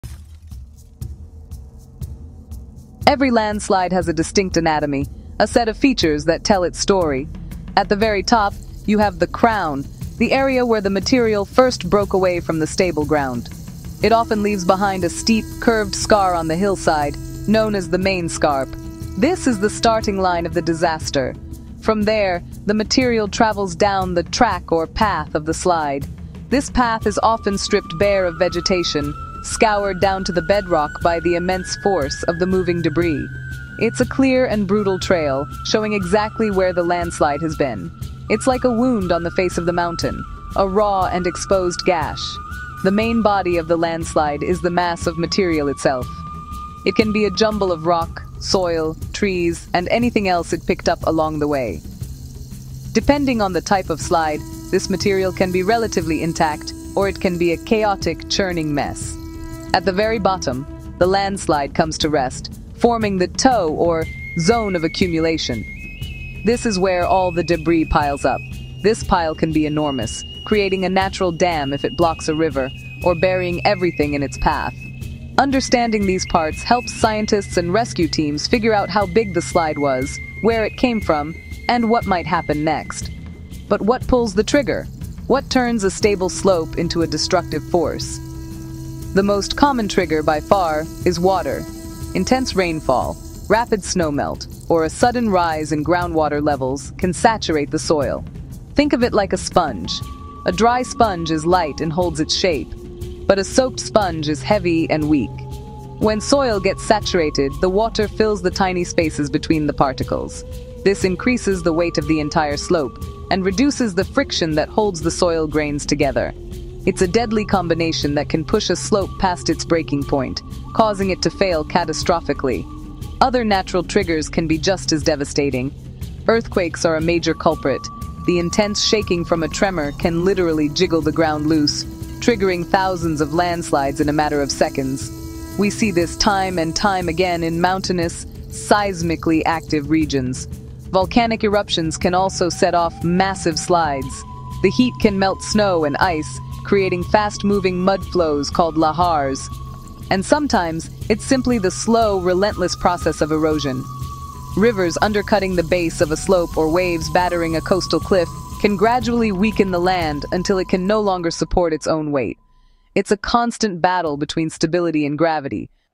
Landslides: Nature’s Unstoppable Force
Landslides are one of nature’s most powerful and destructive processes — capable of reshaping landscapes within seconds. Triggered by heavy rainfall, earthquakes, deforestation, and human activity, landslides remind us of the delicate balance between Earth’s systems.

In this video, we explore:
🌧️ What landslides are and how they occur
🪨 Different types of landslides and slope failures
🌍 The role of climate change and extreme weather
🏞️ How landslides impact ecosystems, infrastructure, and communities
🛠️ Prevention, monitoring, and risk reduction strategies
⚠️ Why awareness and preparedness matter

Understanding landslides helps us reduce risk, protect lives, and plan sustainably in a changing climate.

✨ Nature shapes the land — knowledge helps us live with it safely.